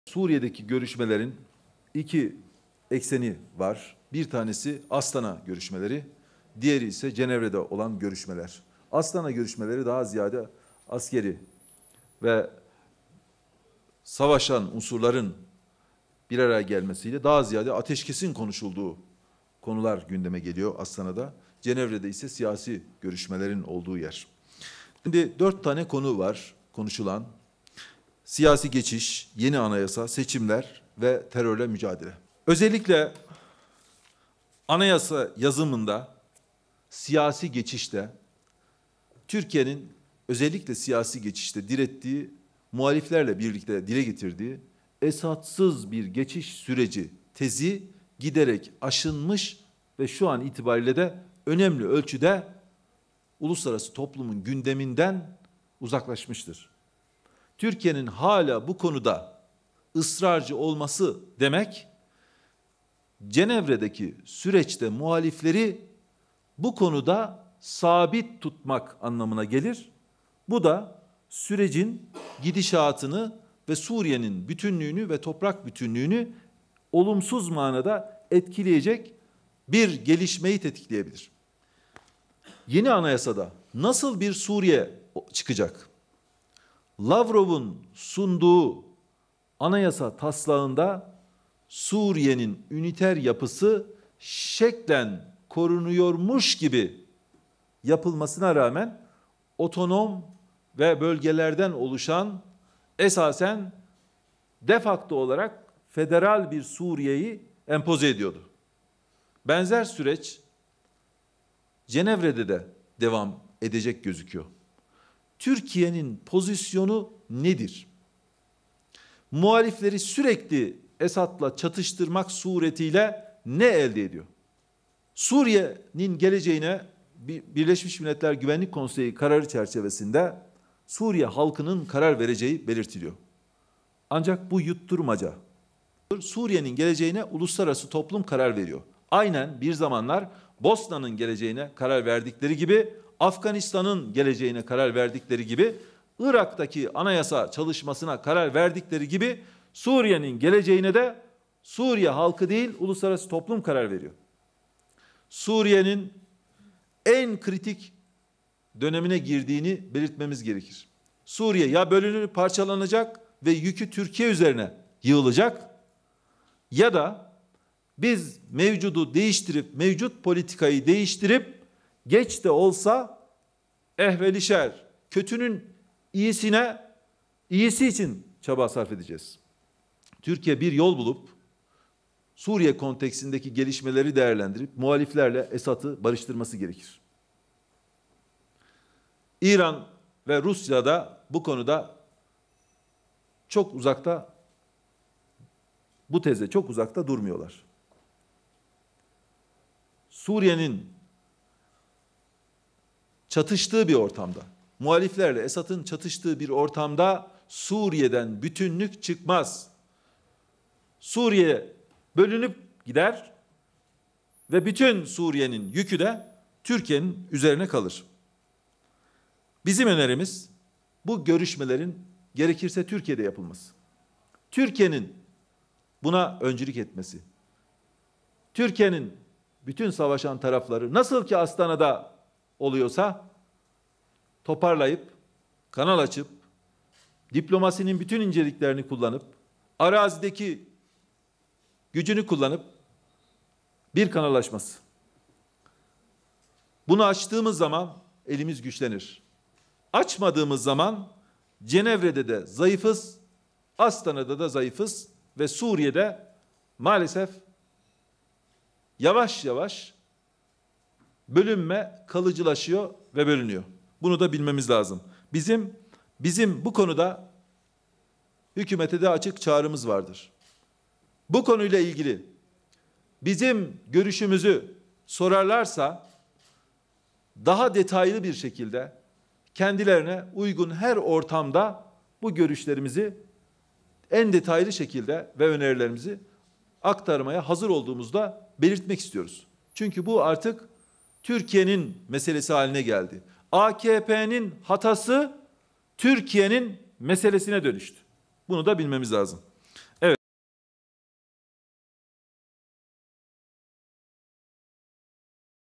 Suriye’nin Bütün Yükü Türkiye'nin Üzerine Kalır!
Genel Başkan Yardımcımız Öztürk Yılmaz: Gerekirse Astana görüşmeleri Türkiye'ye alınmalı. Türkiye’nin bütün savaşan tarafları toparlayıp, diplomasinin bütün inceliklerini ve arazideki gücünü kullanıp bir kanal açması gerek. Bu artık Türkiye meselesi halinde geldi. AKP’nin hatası Türkiye meselesine dönüştü.